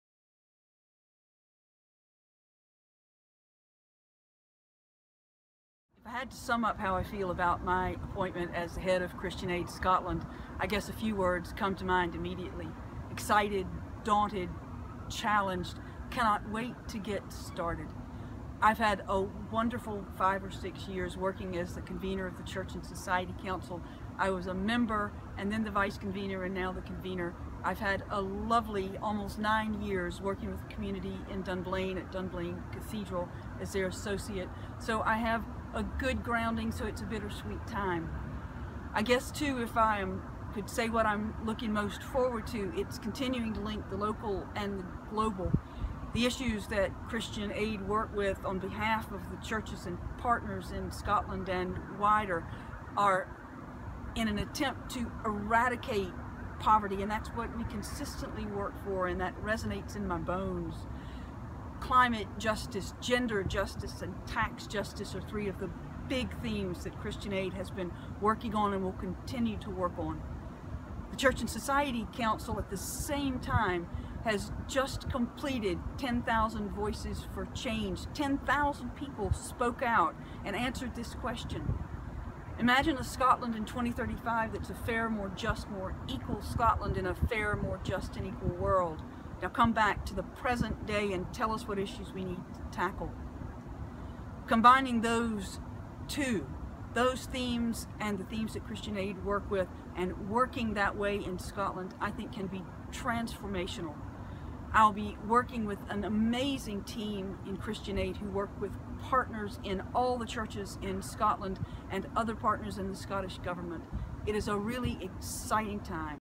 Rev Sally Foster-Fulton appointed head of Christian Aid Scotland
The Convener of the Church and Society Council of the Church of Scotland the Rev Sally Foster-Fulton has been appointed the new head of Christian Aid Scotland.